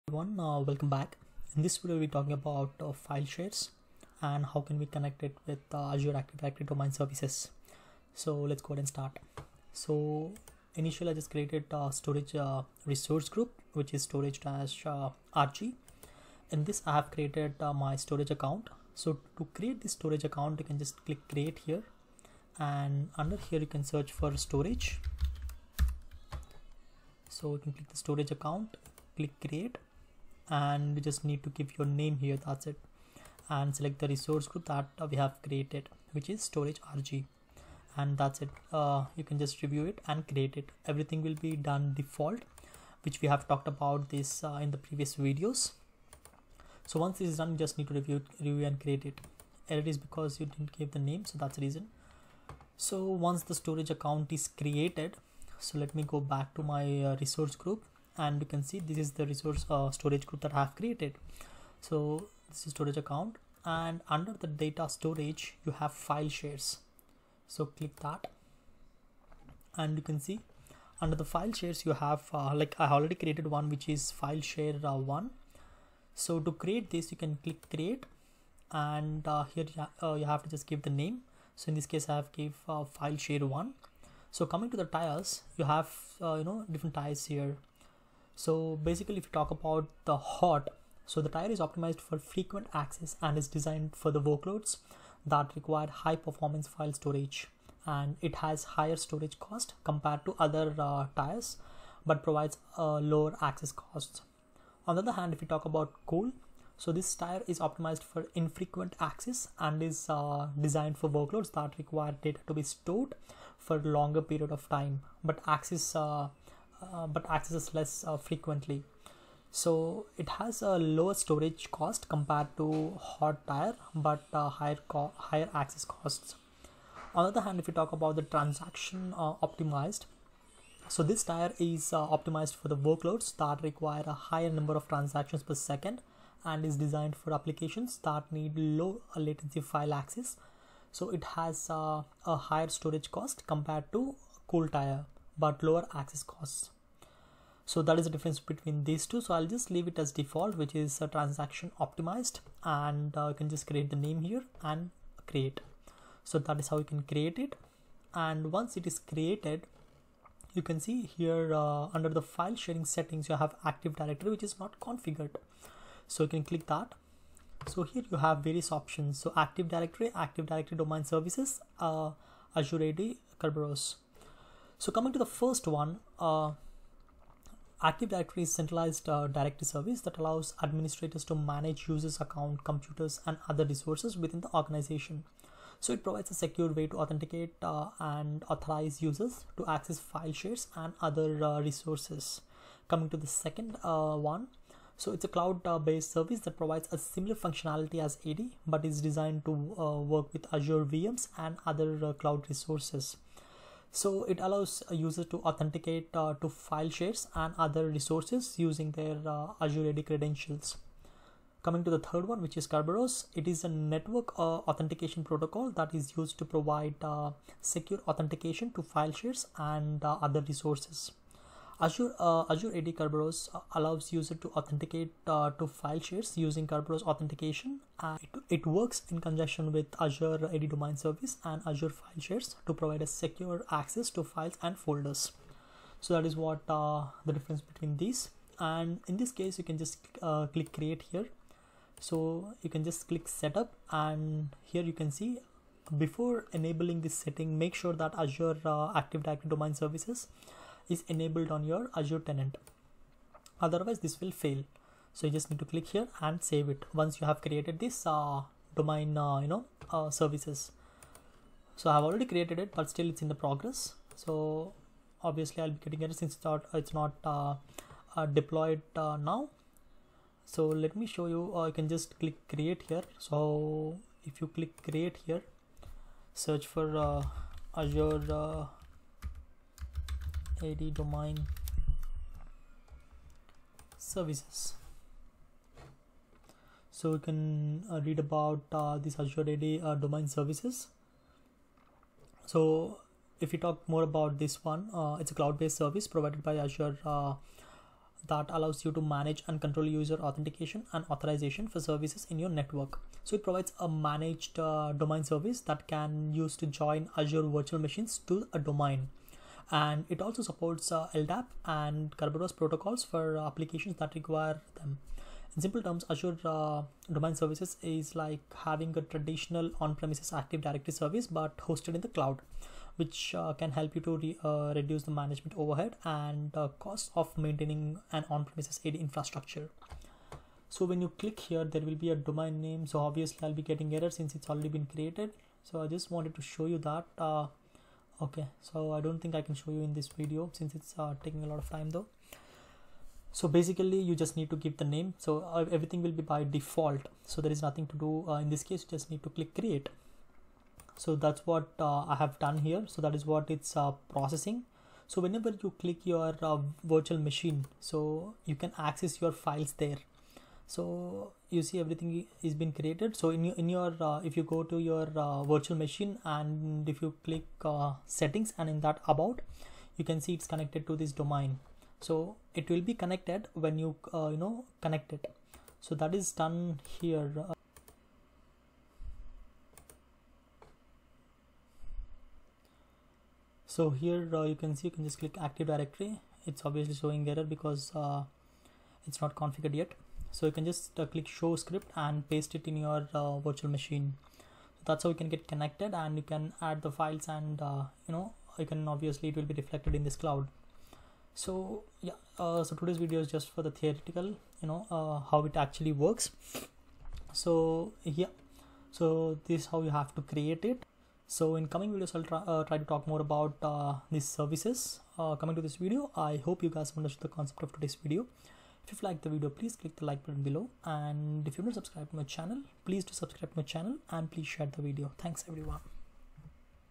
Connecting to Azure File Share using Azure AD Domain Services: A Comprehensive Guide
In this video, we will mostly look into the theory of how can we connect our file share with azure ad domain services.